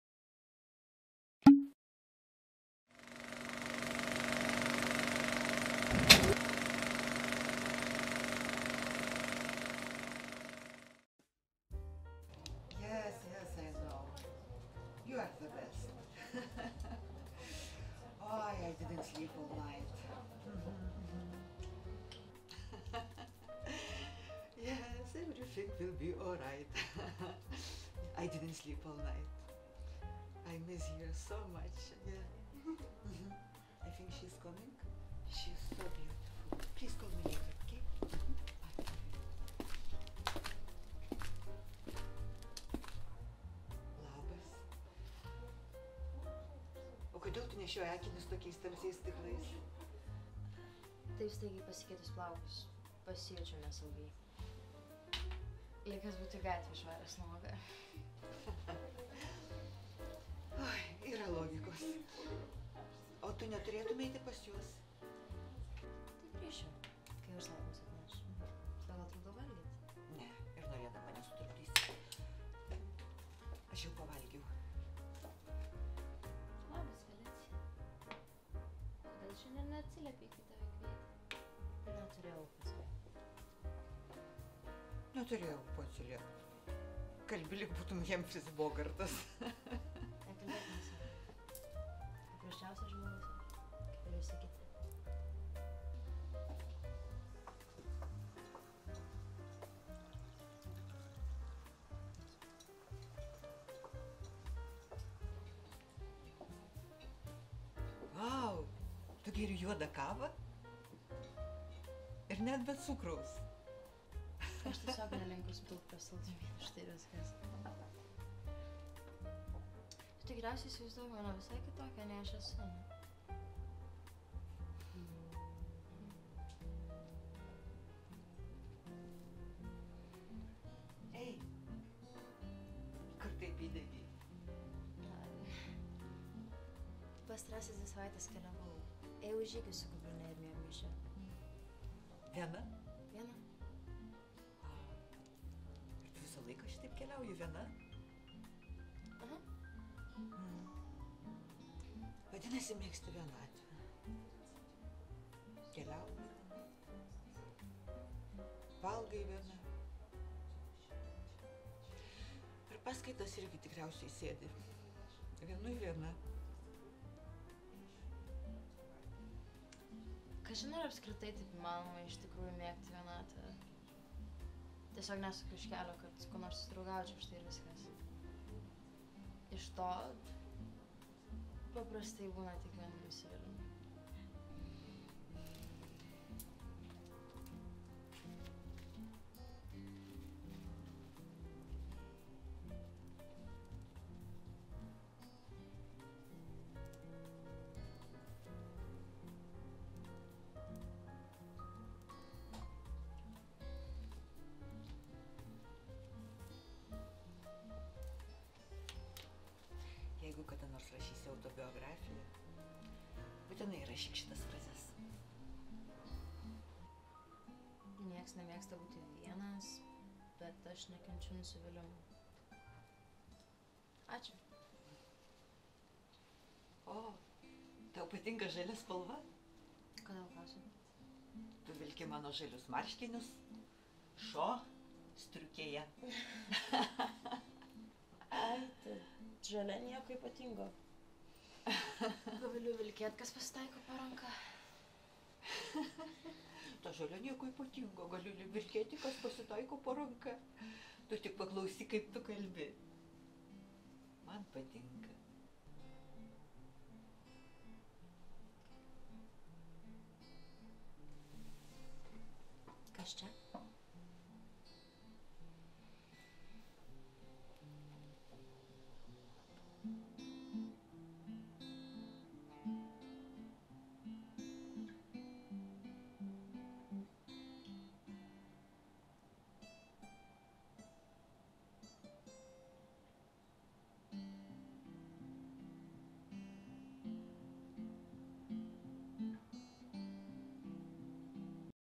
Scenos atkūrimas
II režisūros kurso studentės Julijos Sotirovič rudens semestro darbas. Užduoties pavadinimas - scenos atkūrimas. Darbo vadovės - Aneta Bublytė ir Dovilė Šarutytė. 2023 metai.

Projektą remia Lietuvos kultūros taryba